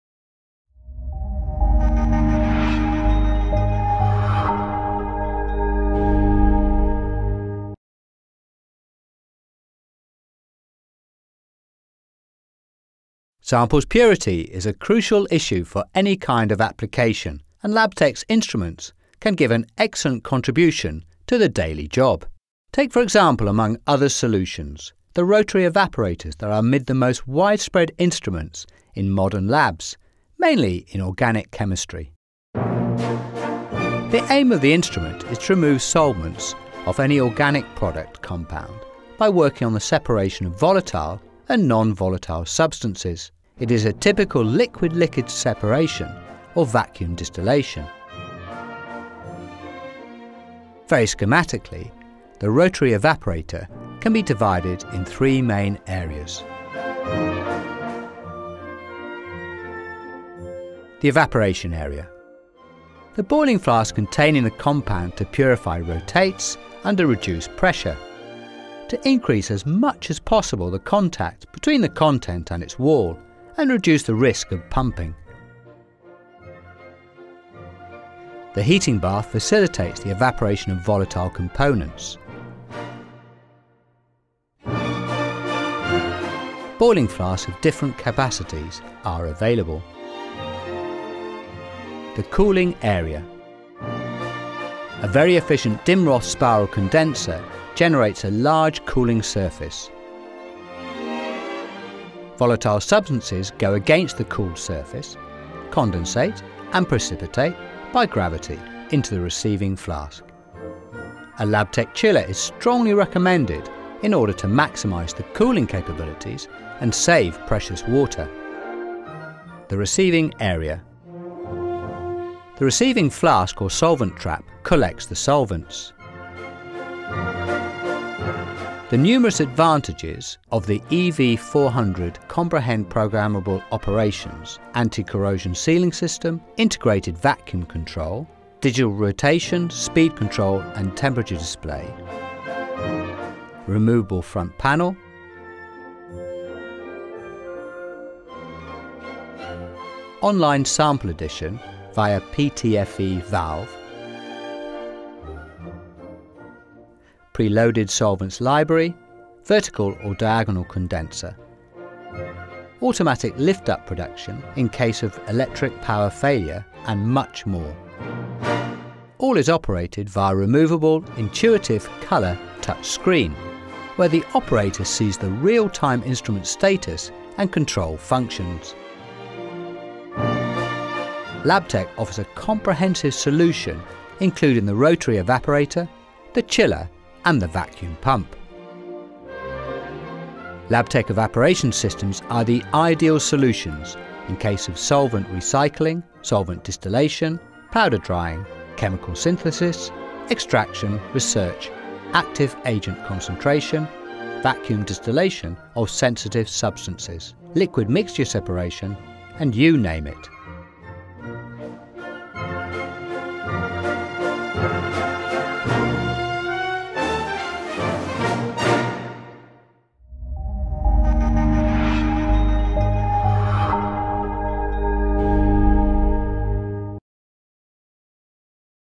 LabTech - Rotary Evaporator EV 400 VAC - evaporation solution
The LabTech evaporation solutions introduce a new concept in routine distillation/evaporation processes: safety, efficiency and durability to support the lab user work! With an innovative and ergonomic design, the EV series rotary evaporators include color digital display to monitor and control rotation speed and temperature, motorized vertical lift, evaporation flasks, on line sample adding through PTFE valve and can be completed with the LabTech vacuum and cooling accessories to reach top effi ciency of process. The stand alone heating bath guarantees maximum safety in operations and easy maintenance.